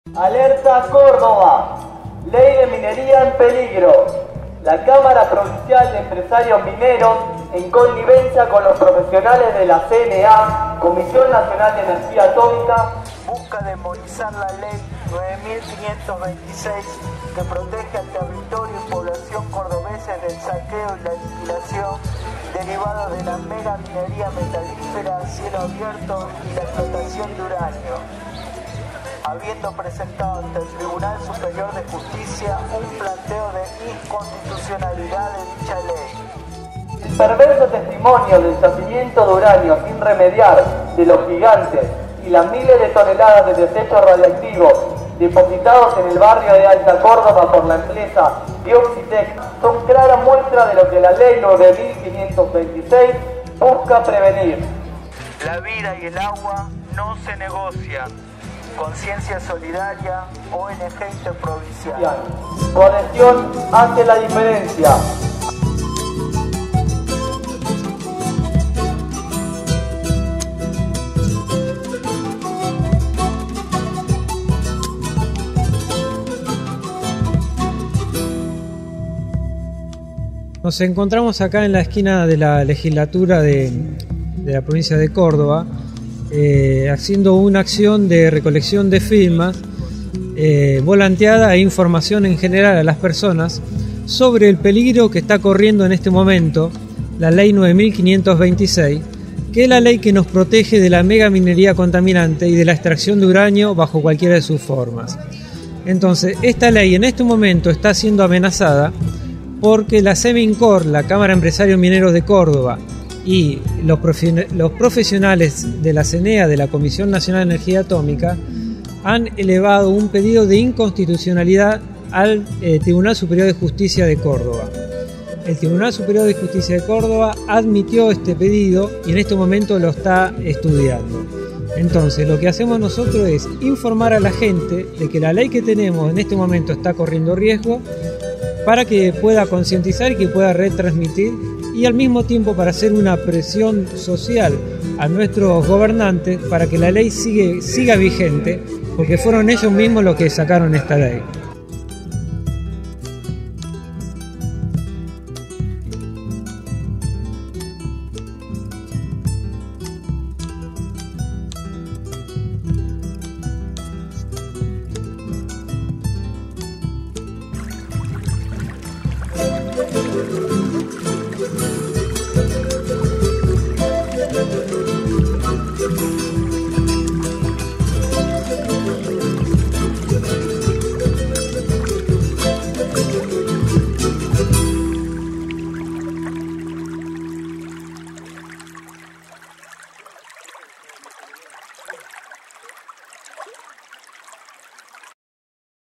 La ley 9526 de prohibición megaminera es Constitucional y de la Ciudadania
Firma para que la ley de prohibición de la minería metalífera a cielo abierto de la provincia de Córdoba siga vigente

En 2008, casi por unanimidad, la Legislatura Cordobesa llevó al pueblo cordobés a la tranquilidad votando la Ley 9526 que nos protege de la megaminería a cielo abierto con métodos de lixiviación, con productos químicos de alta toxicidad, como el cianuro y el ácido sulfúrico entre otros, así como la prohibición de explotación de uraNiO y torio bajo cualquiera de sus formas.
Hoy esta ley, ejemplo para otras provincias y países, se encuentra cuestionada por los intereses oscuros de la CNEA y la CEMINCOR.

El pueblo todo tiene puestos sus ojos sobre el Honorable Tribunal Superior de Justicia, esperando deseche el injusto pedido de inconstitucionalidad que pesa sobre la Ley 9526. Sabemos que una decisión histórica está en sus manos, y el apoyo del pueblo al rechazo del TSJ sobre la inconstitucionalidad es manifiesto.
ONG Conciencia Solidaria, así como distintas entidades populares y asambleas, nos encontramos en constante alerta y movilización.
No permitiremos que Córdoba se convierta en otras San Juan o Catamarca.
 REITERAMOS NUESTRO APOYO A UN FALLO QUE SEA JUSTICIA
y que nos permita apreciar un Poder Judicial independiente, al cual poder confiar la vida y los intereses del pueblo cordobés.
Para muestra tenemos la macabra mina de uraNiO de Los Gigantes y su irremediable pasivo ambiental.
       Decimos  SÍ   a la   Minería   Tradicional   Cordobesa